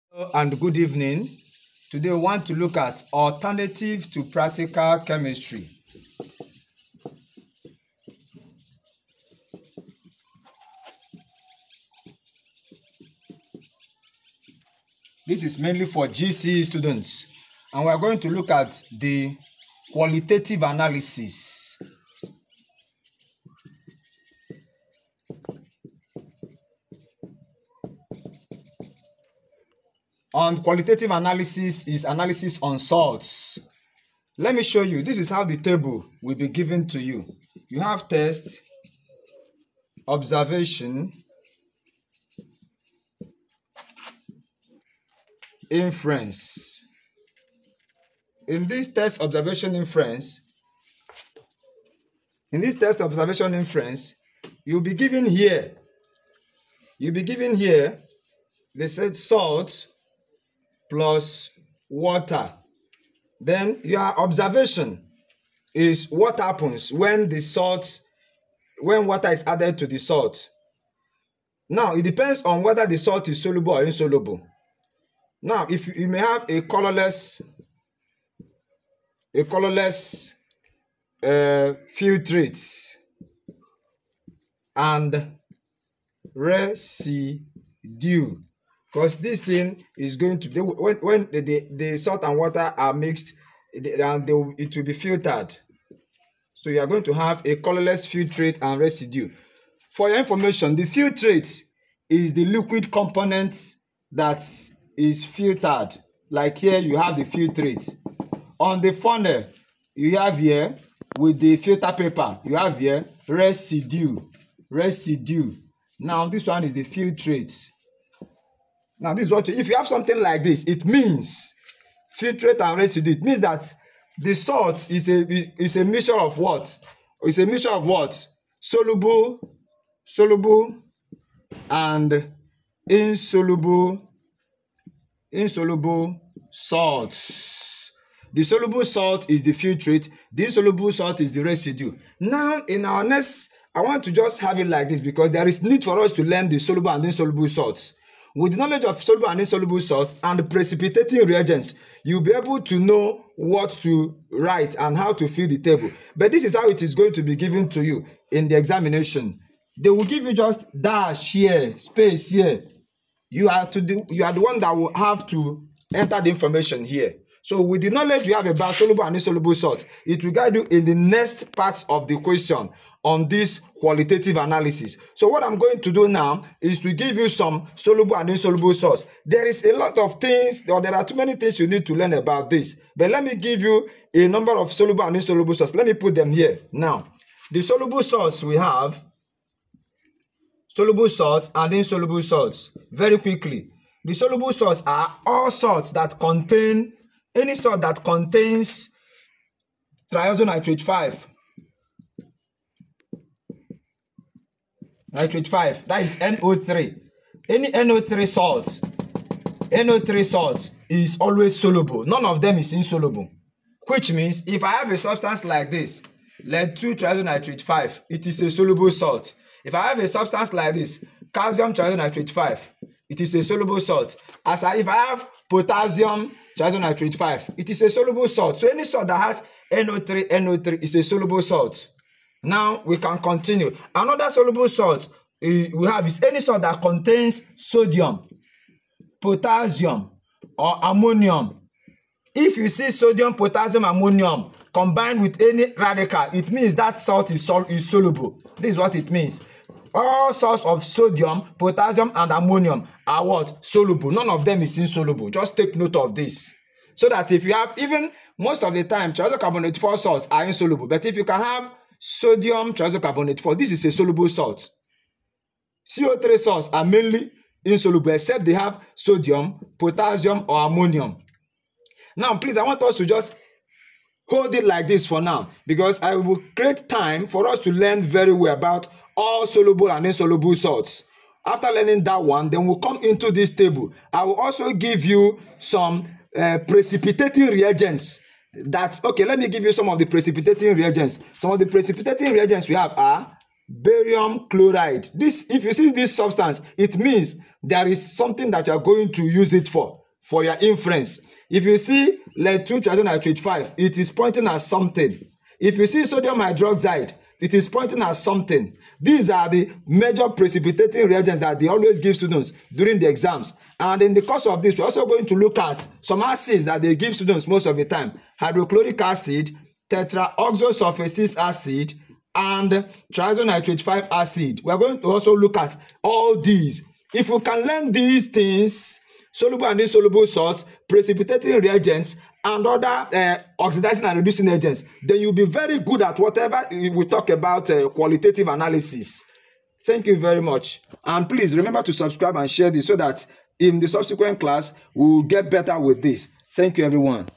ALTERNATIVE TO PRACTICAL CHEMISTRY (1). GCE 2023. THE SECRETS AND WHAT YOU MUST KNOW
This is a foundation for success in QUALITATIVE ANALYSIS for all science students.
Join us as we bring you a FULL TEACHING on the topic.
You can always do it YOURSELF after learning with us.